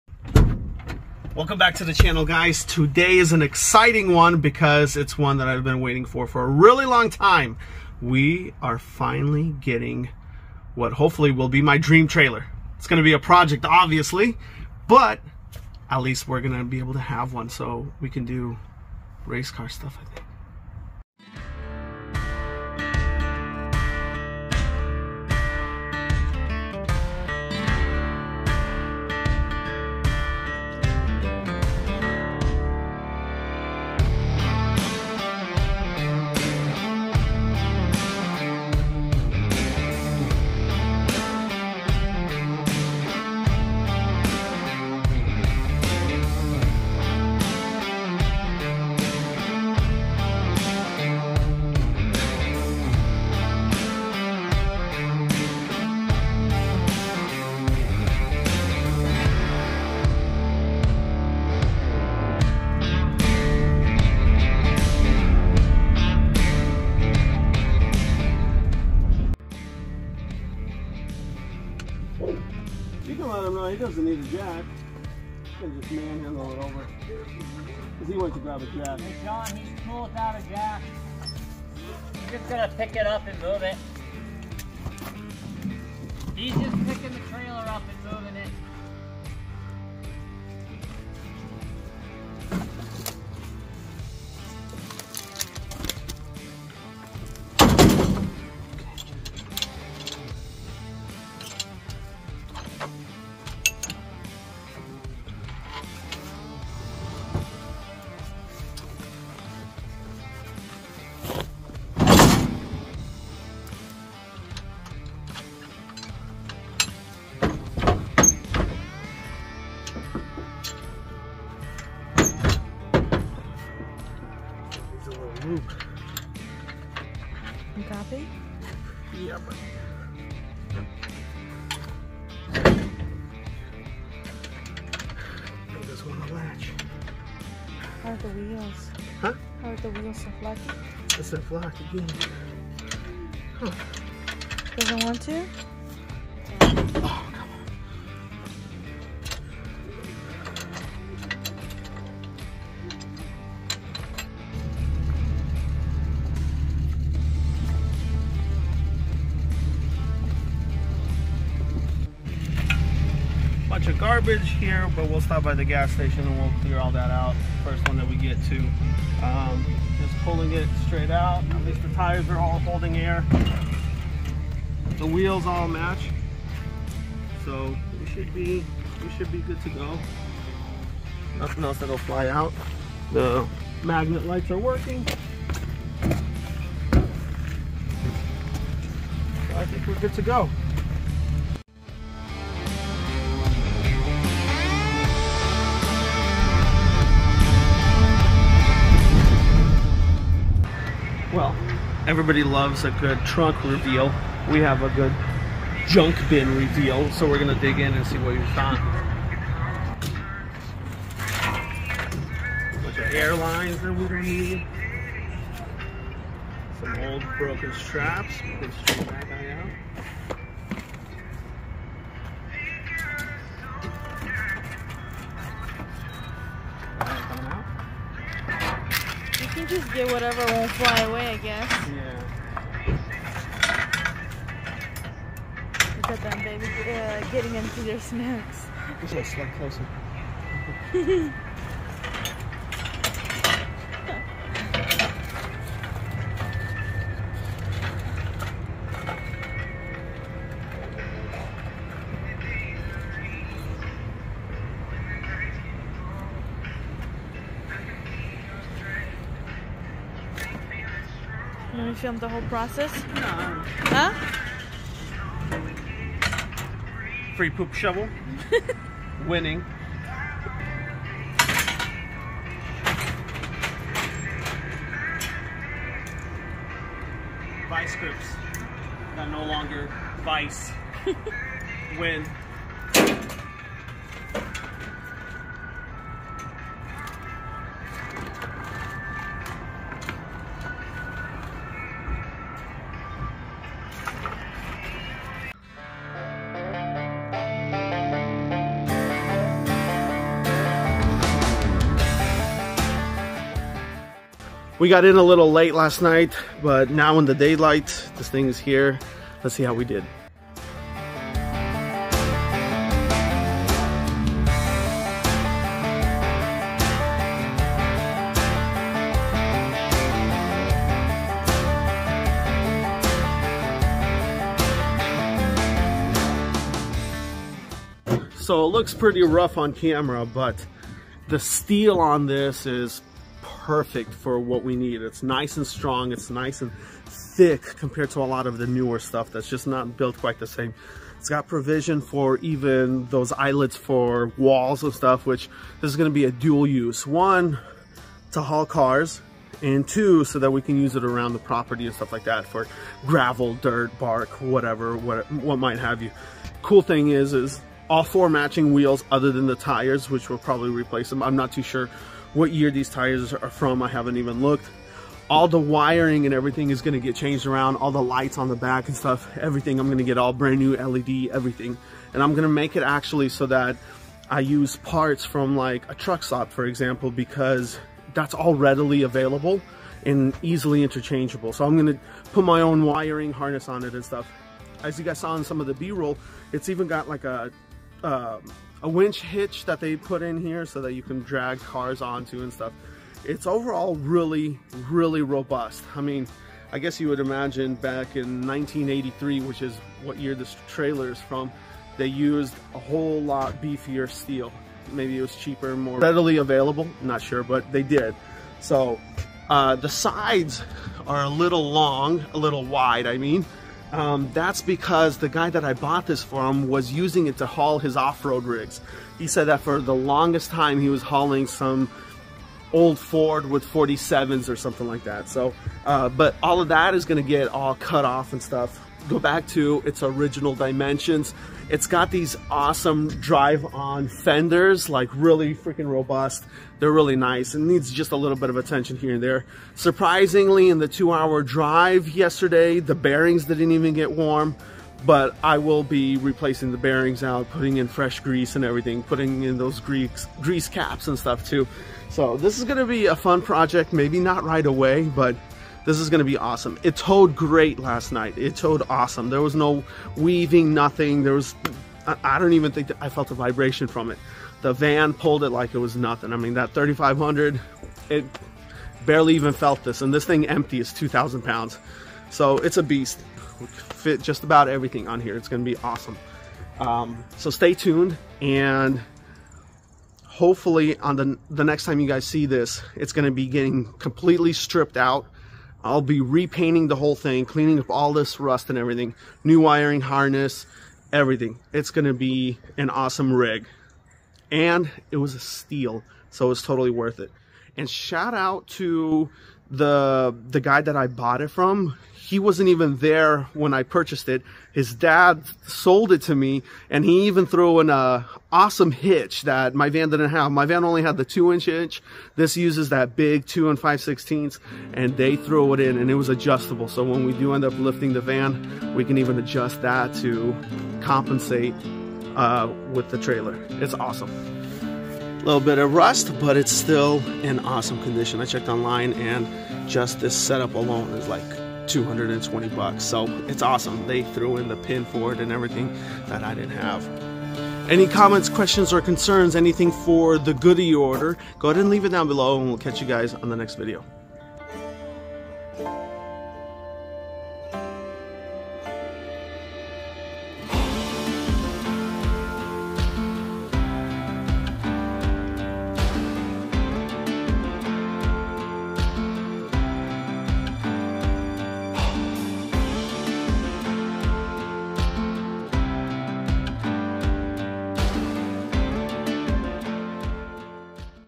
can we tow the CHEAPEST! 22ft titled car hauler on the internet 200 miles home for restoration?
found this on Facebook marketplace for $900, when we got to the seller, the ball hitch I had was too small, so he gave me his hitch set up, I would say valued around $200 making this trailer only $700! rolling and all, we head back down 200 miles to home.

follow us on the social media's were we try to post previews of all the builds and videos we're working on.